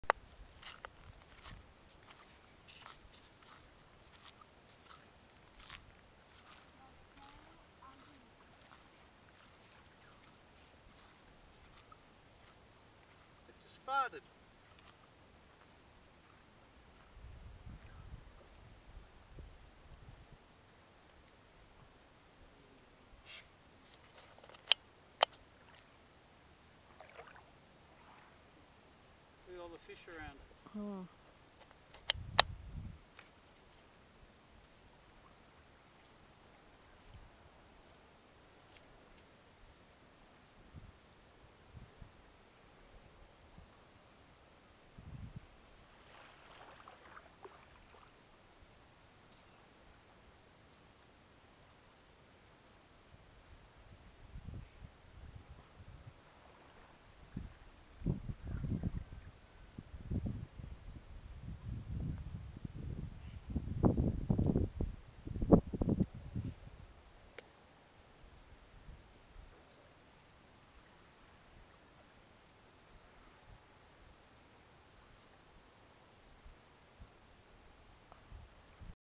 Estuary Stingray, Dasyatis fluviorum
An Estuary Stingray feeding in the shallows at Hare Bay, Jervis Bay, New South Wales, 23 April 2014.